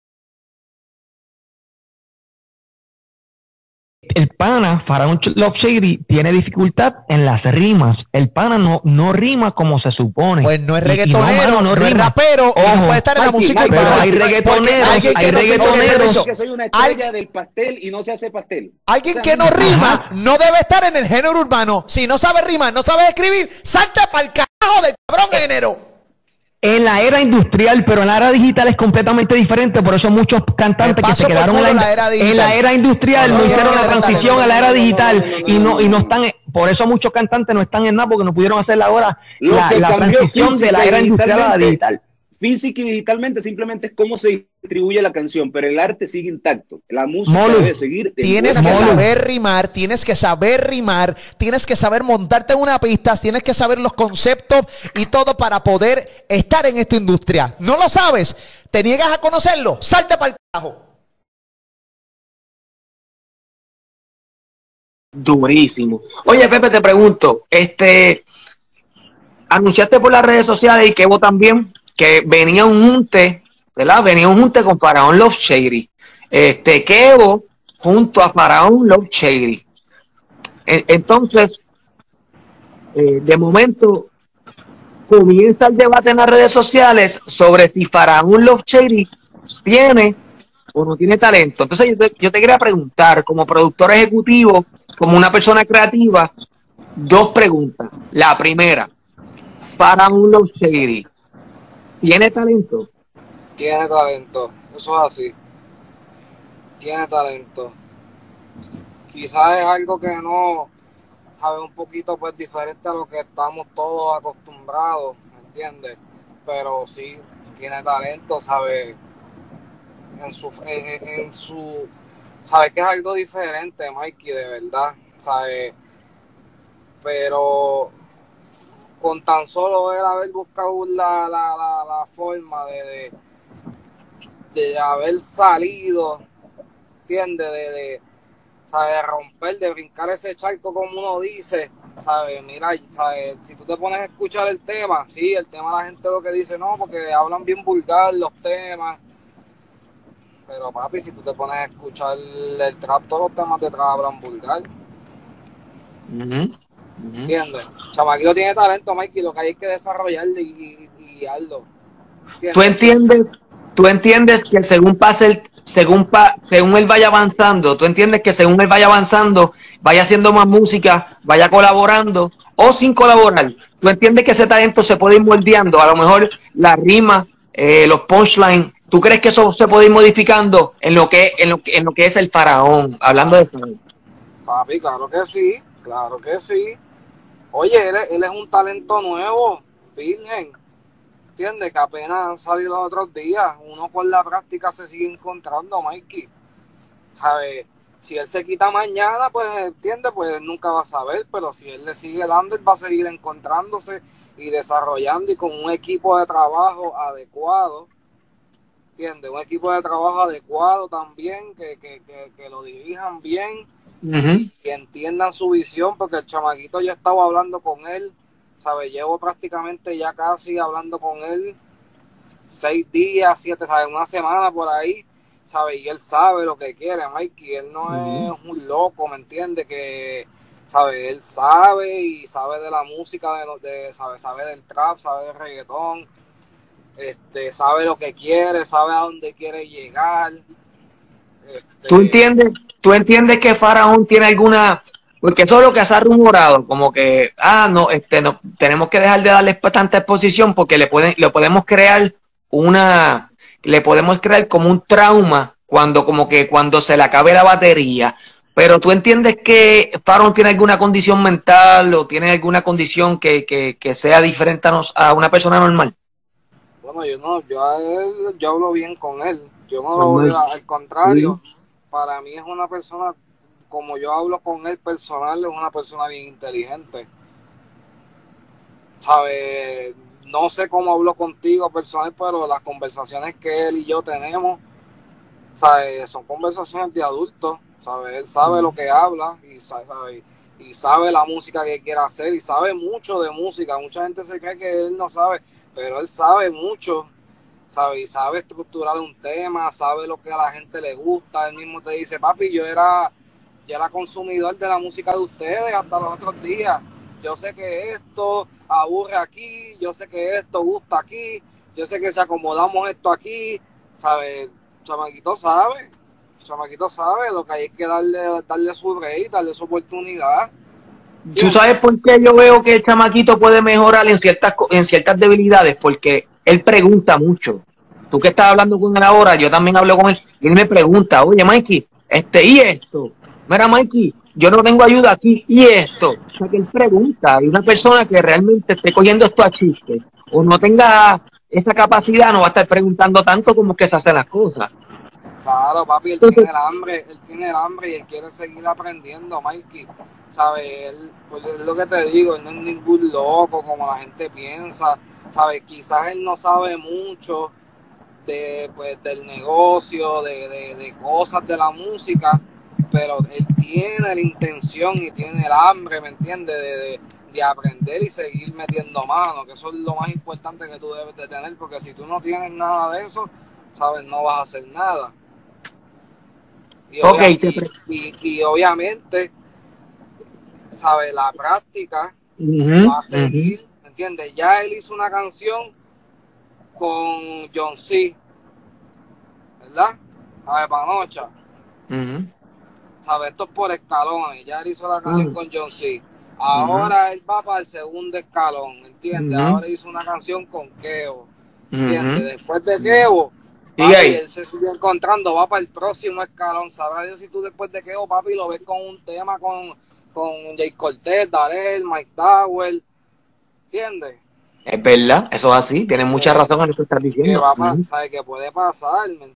Kevvo, Pepe Quintana y Maiky Backstage defienden a Faraon Love Shady y dicen que si tiene talento
Maiky Backstage y Pepe Quintana defienden a Faraon Love Shady y dicen que si tiene talento